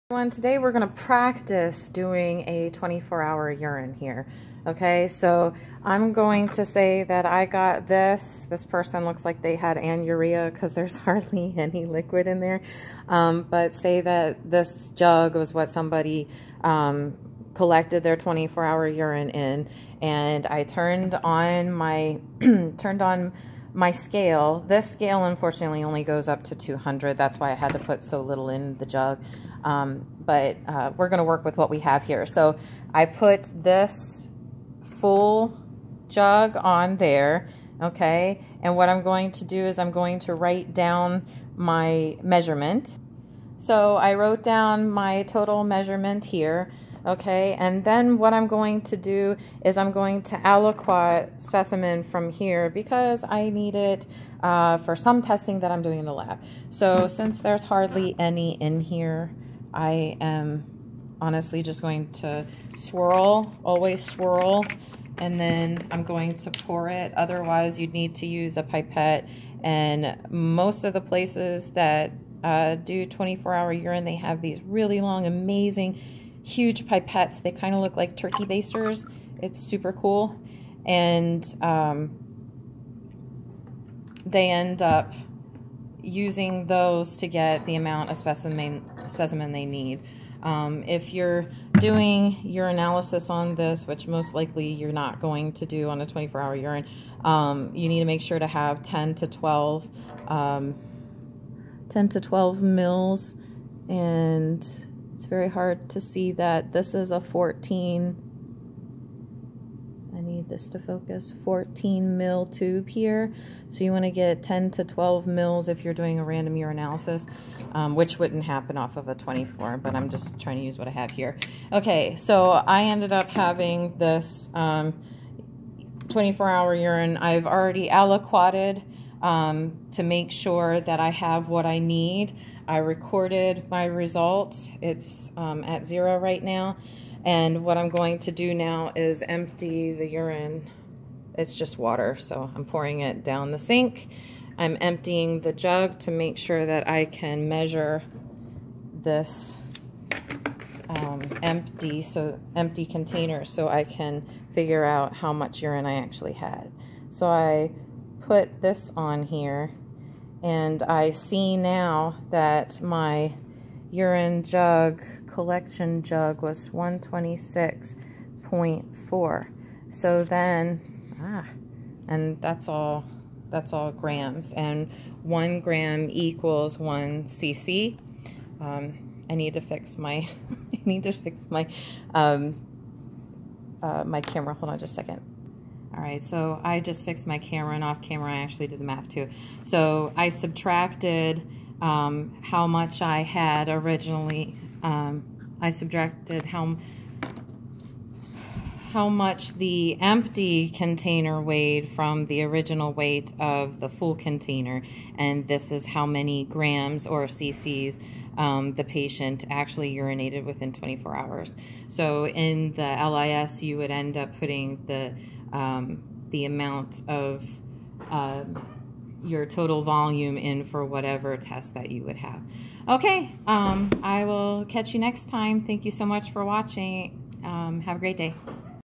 Urinalysis: Example of aliquoting a 24 hour urine
This video shows an example of aliquoting a 24 hour urine and the math involved.  The video is an extension of the "Urinalysis: Different Types of Urine Specimens" video.